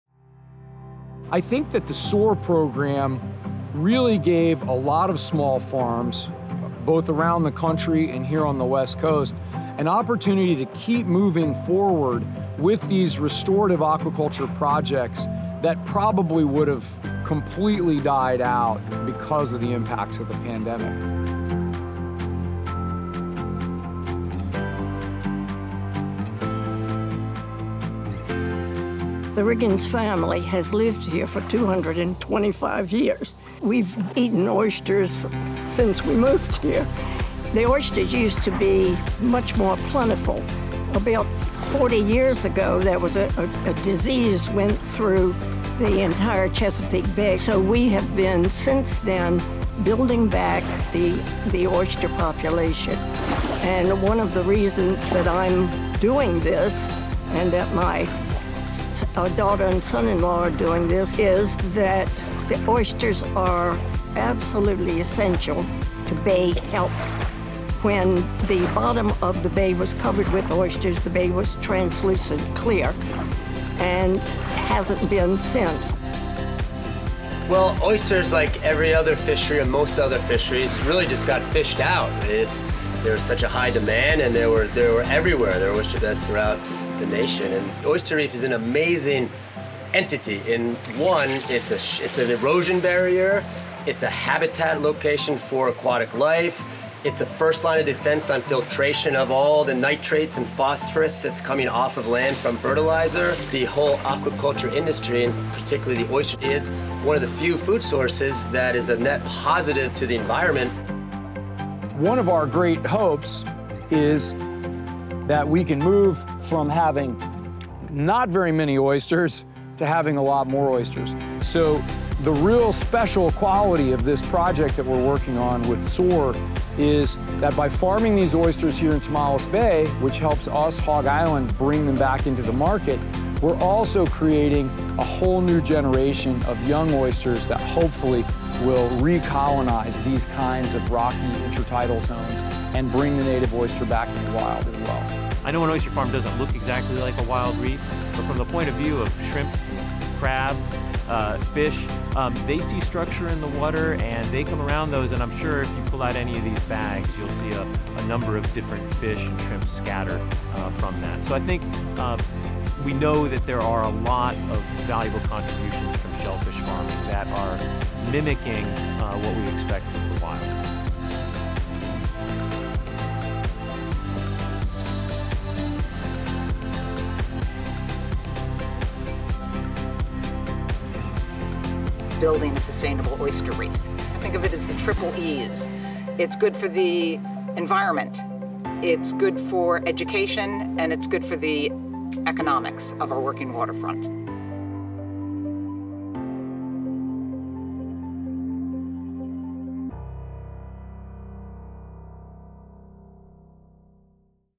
A SOARing Future for Coastal Communities and Ecosystems
Oyster farms support coastal jobs and economies, provide an important food source, and improve water quality – but the industry is facing big challenges. In partnership with The Pew Charitable Trusts, The Nature Conservancy is paving the way for a more resilient and sustainable shellfish industry that benefits the ocean and the communities that rely upon it.